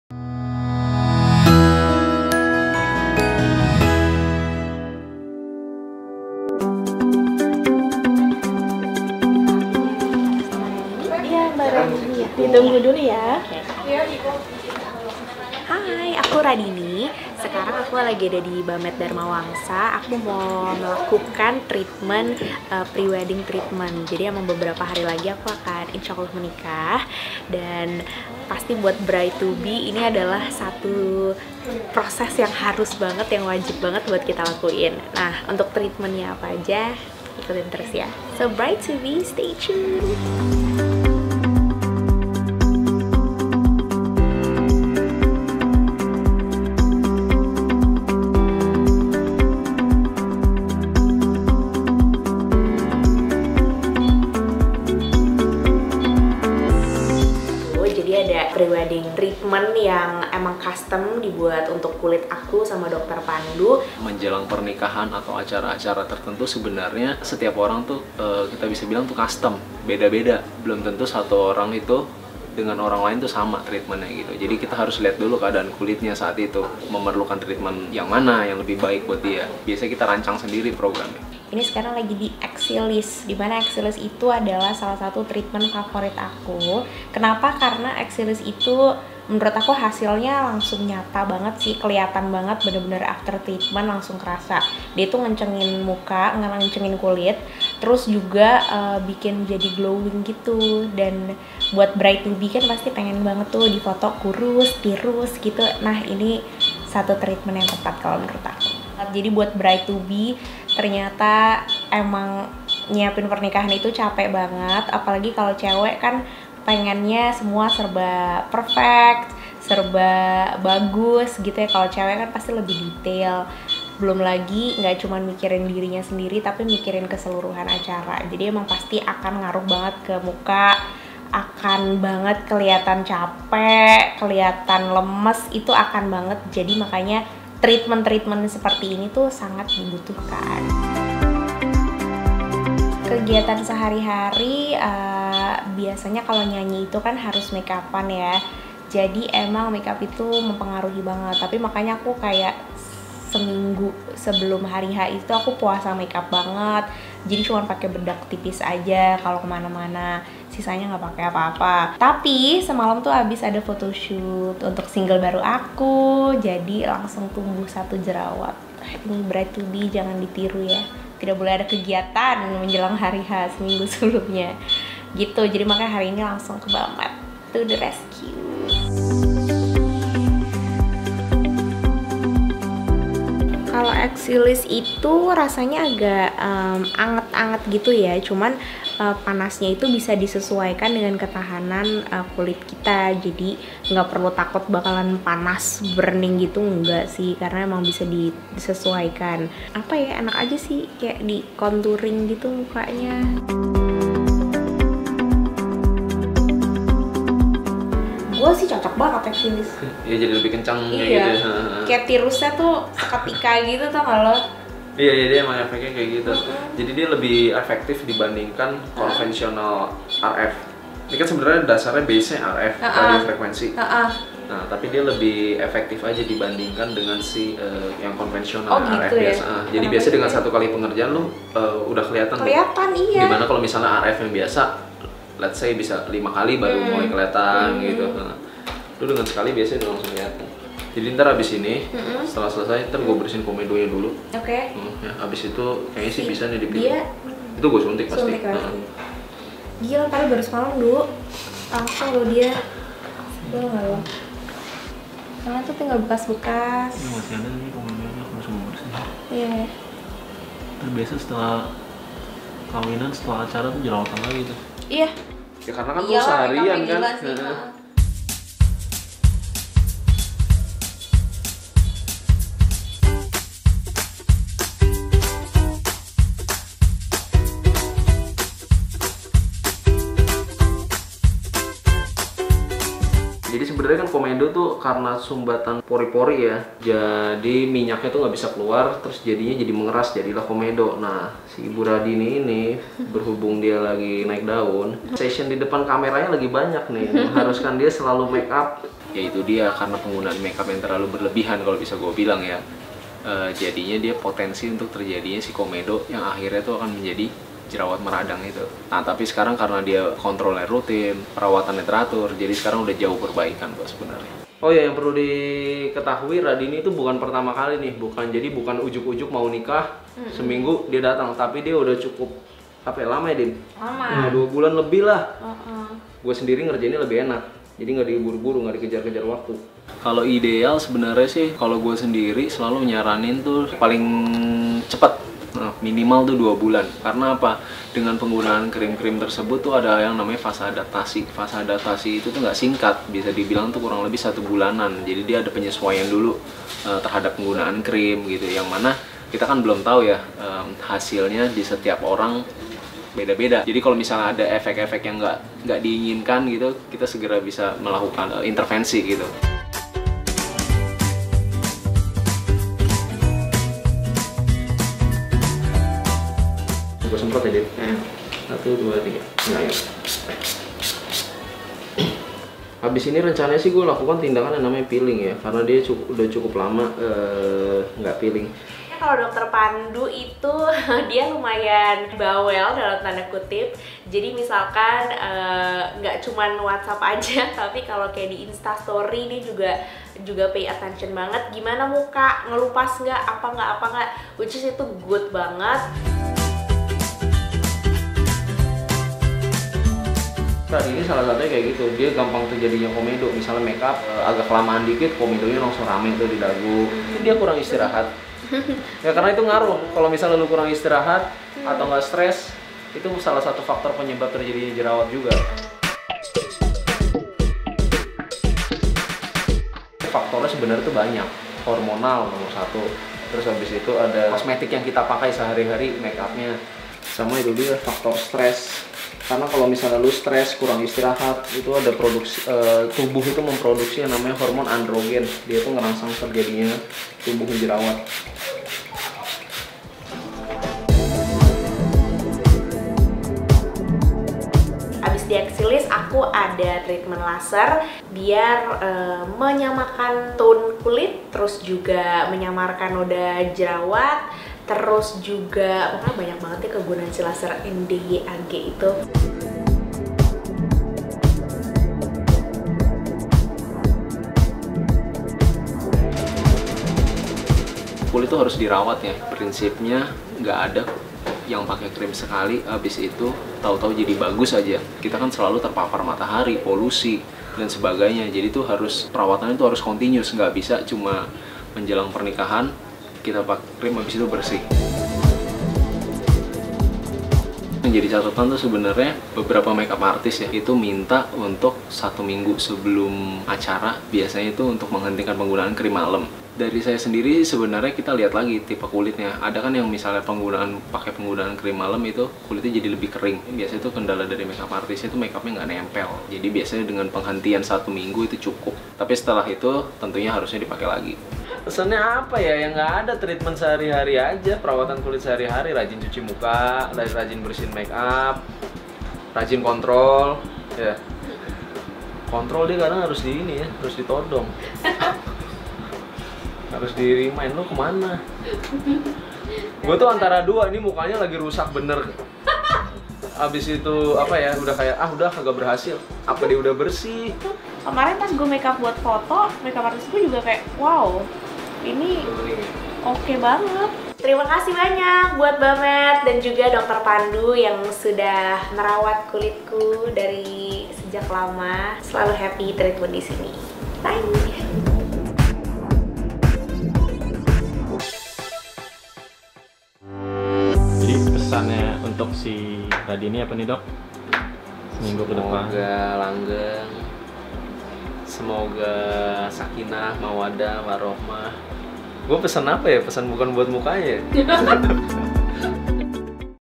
Radhini's Prewedding Treatment | Bamed Vlog
Of course, we've already heard about Radhini's intimate wedding ceremony. But there's a panic story, precisely a week before the Big Day. Her acne prone skin relapsed after finished her new song-album cover photo session. Luckily, this multi-talent belle had dr. Pandu Pradana to the rescue! while having pre-wedding treatment at the same time. Let's see her myBAMEDskin!

BMS PRO is a creative media specialist, subsidiary of Bintang Group

© BMS PRO & Bamed Health Care. 2018.